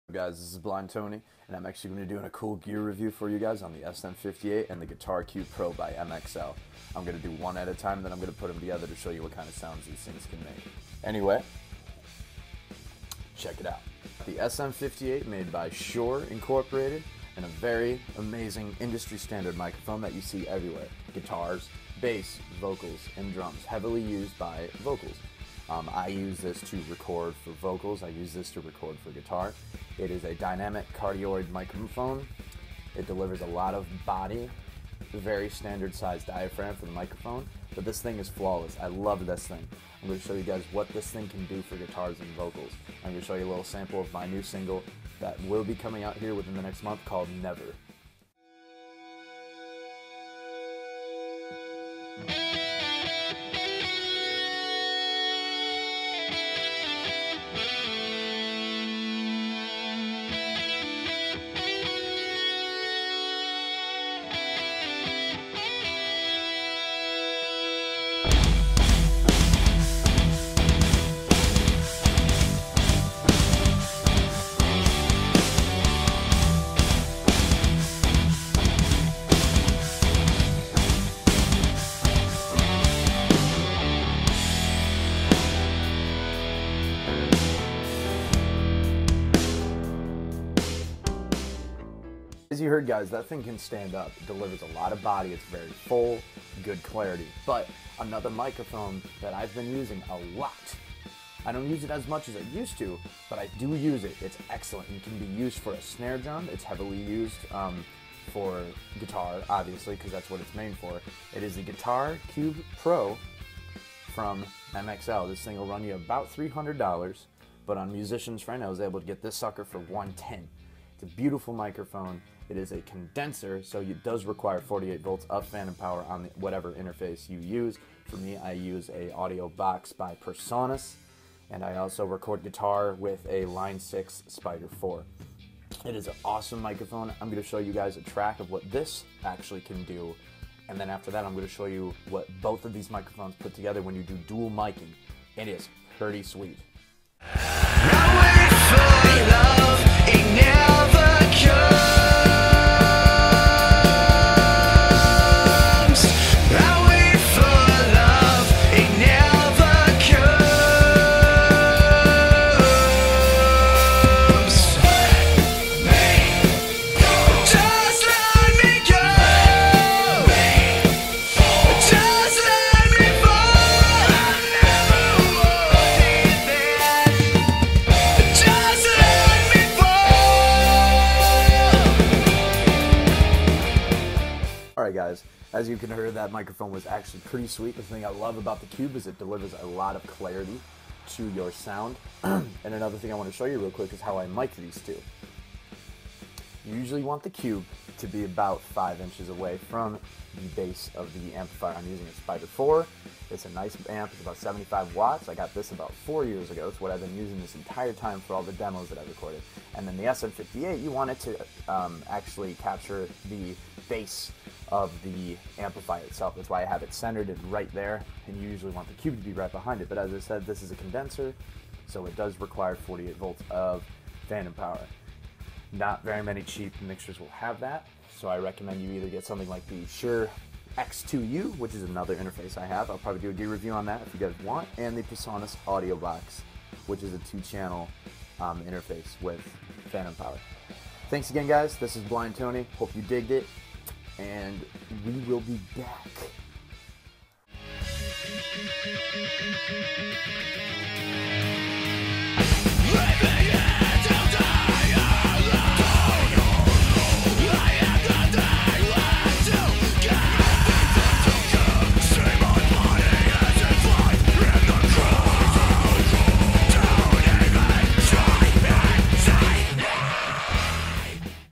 Sm58 VS Guitar Cube Pro -- Microphone Review
Sm58 from Shure vs the Guitar Cube Pro by MXL. Both microphones are very useful for recording your instrumentals.